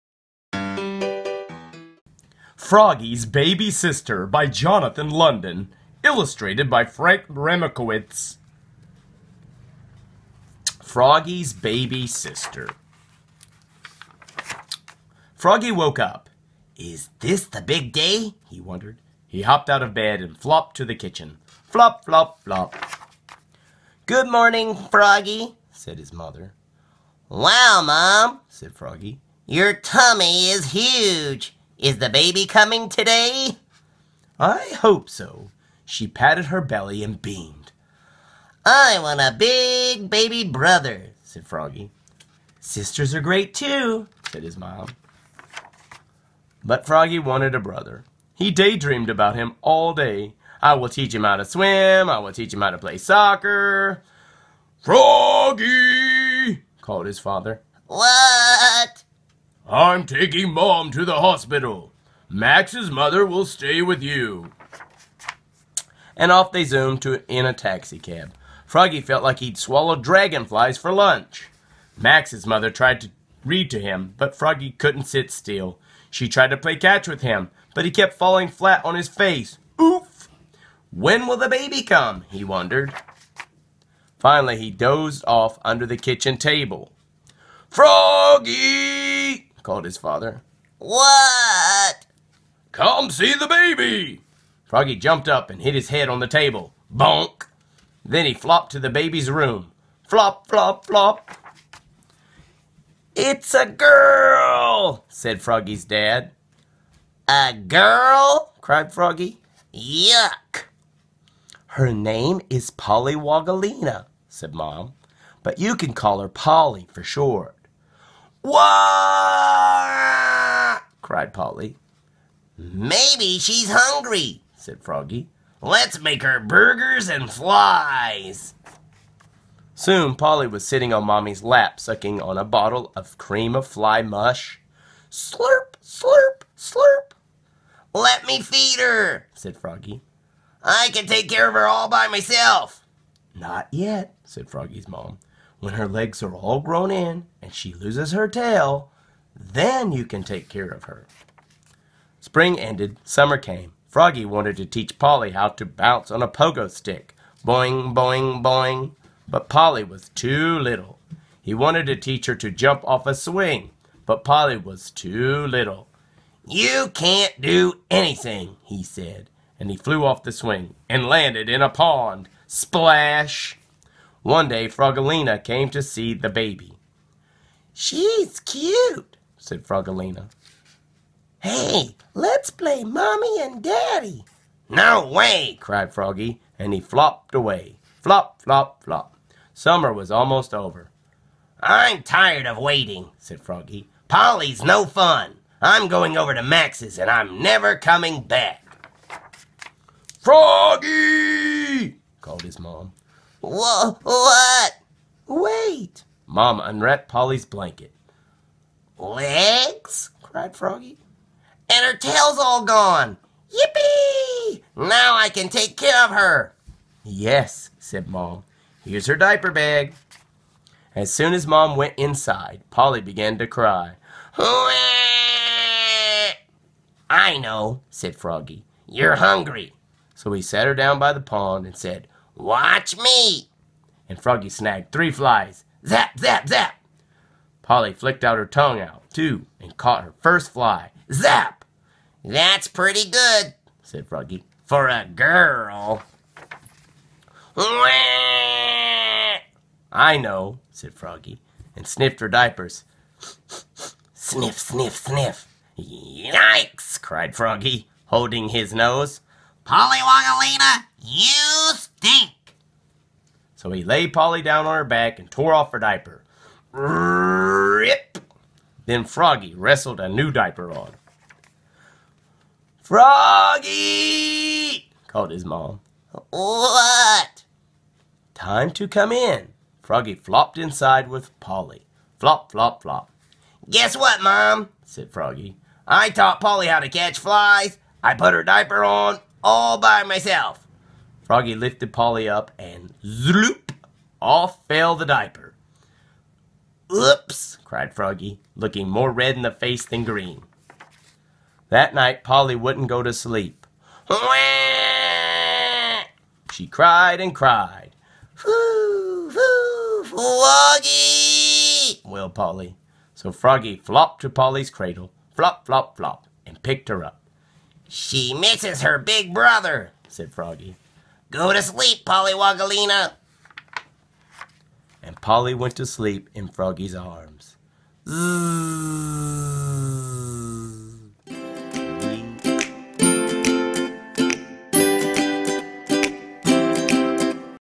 Study Cubby - Froggy's Baby Sister Books Read Aloud
Study Cubby reads "Froggy's Baby Sister" by Jonathan London.  Froggy's parents are expecting a new baby, and Froggy is hoping for a brother. So when his parents introduce him to Pollywogilina, his new baby sister, Froggy is disappointed. Not only is she a girl, but Polly is too little to do anything! Froggy isn't even allowed to take care of her until her tail falls off. Waiting is hard, but just when he's ready to give up, Polly's legs finally grow and big brother Froggy steps in to show her the ropes.